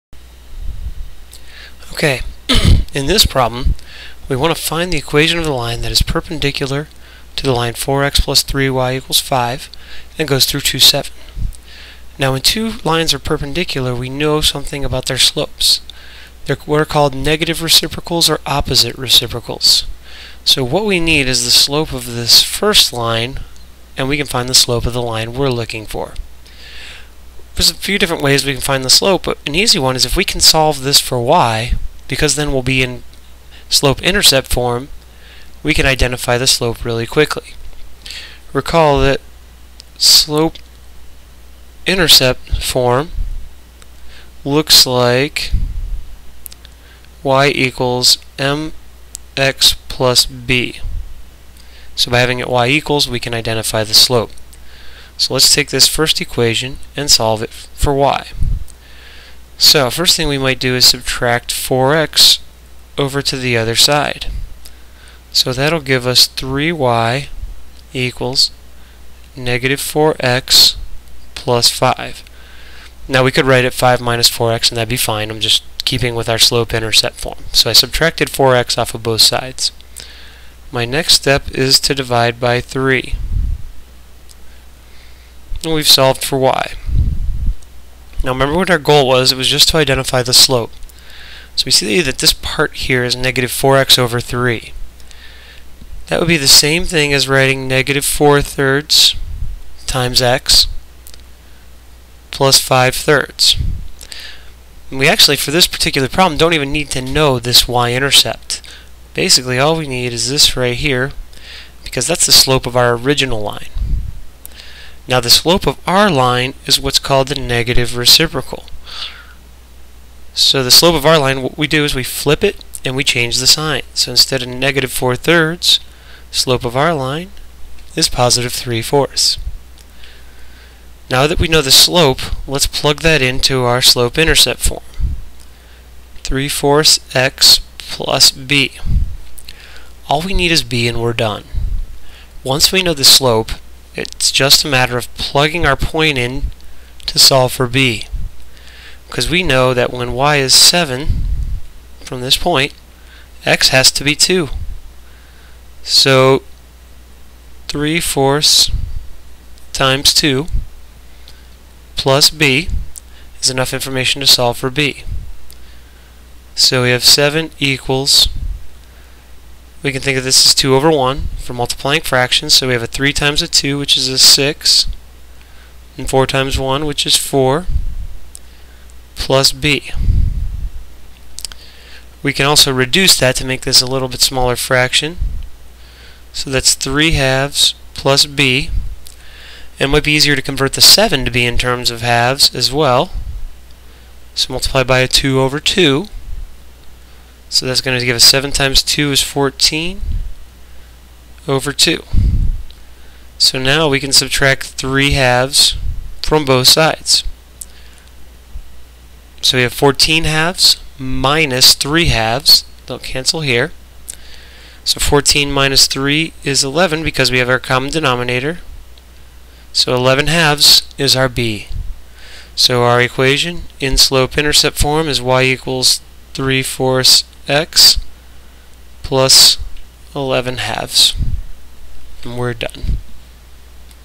Finding the Linear Function Perpendicular to a Given Line
In this video we find a linear function that is perpendicular to a given line.